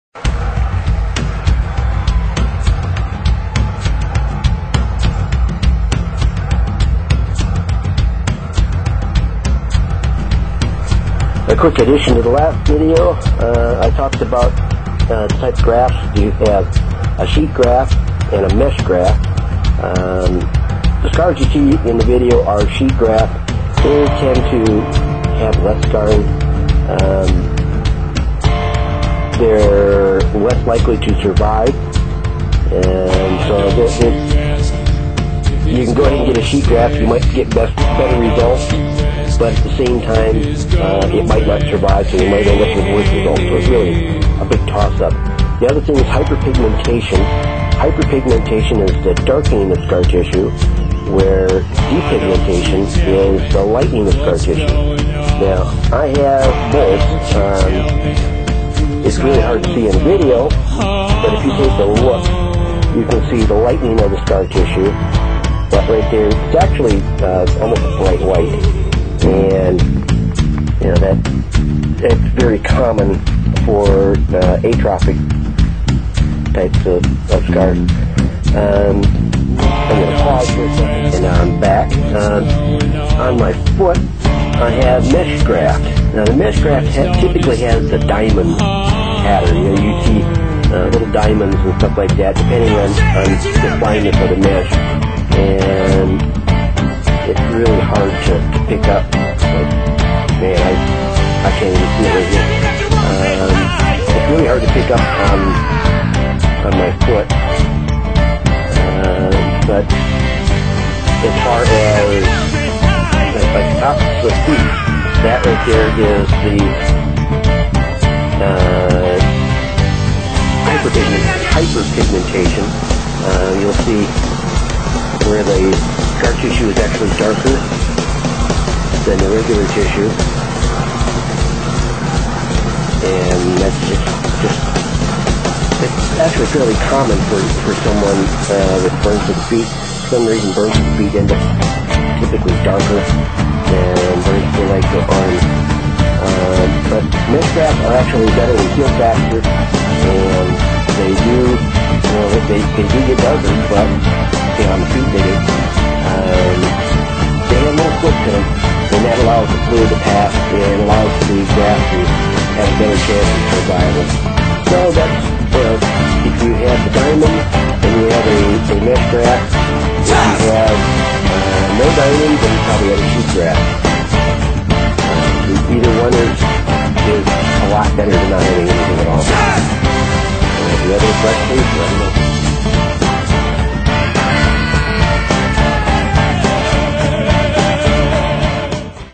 All about burn injuries and burn scars (part II)
this is part two on skin grafts and burn scars.